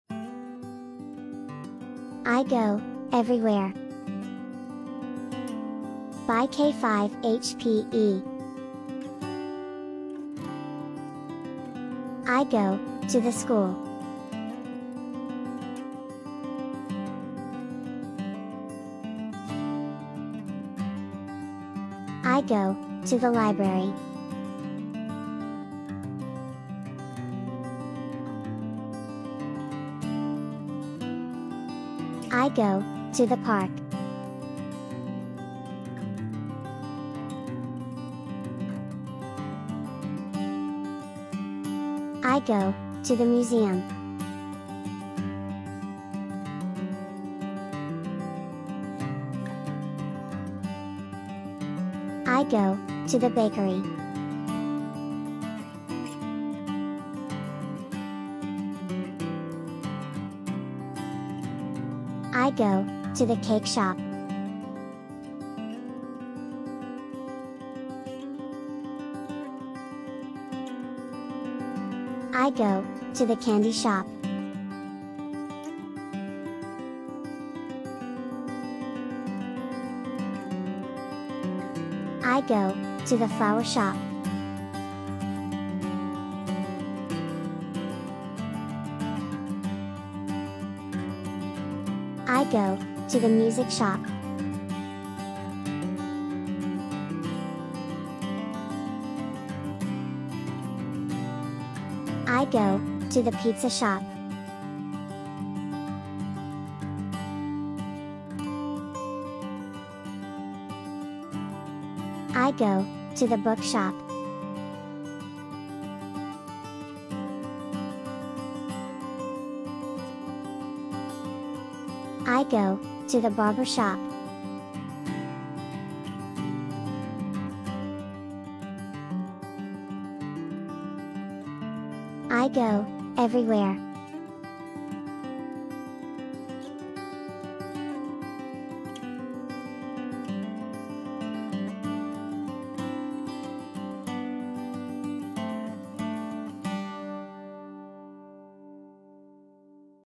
I Go Everywhere Sight Word Reader, Places in the Community Vocabulary, Early Literacy Learn to Read!
This is a short Video which features sight words (I, go, the, to) and a collection of different places in the community.

Sight words are words that should be memorized to help a child learn to read and write. Learning sight words allows a child to recognize these words at a glance — on sight — without needing to break the words down into their individual letters and is the way strong readers recognize most words. Knowing common, or high frequency, words by sight makes reading easier and faster, because the reader does not need to stop to try and sound out each individual word, letter by letter. The best way to learn sight words is through lots and lots of repetition, and in the form of flashcard exercises.